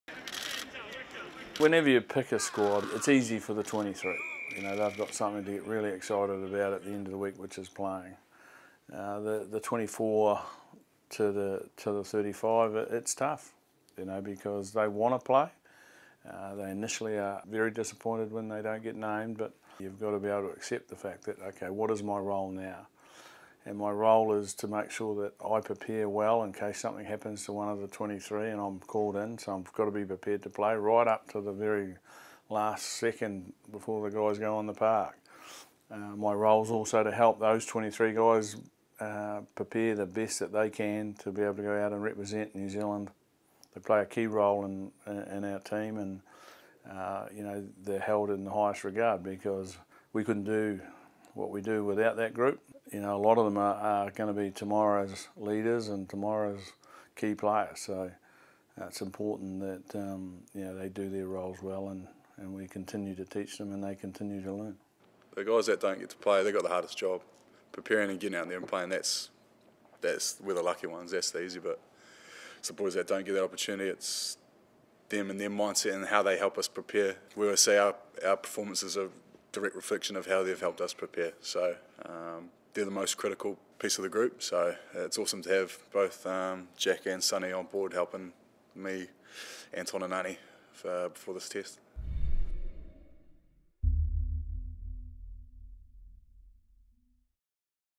All Blacks wider squad has big role to play
With an unchanged lineup to face France in the second Test of the Steinlager Series, coach Steve Hansen and second five-eighth Ryan Crotty explain the big role played by those not selected.

Limited tickets are still available to the final Test of the Steinlager Series in Dunedin.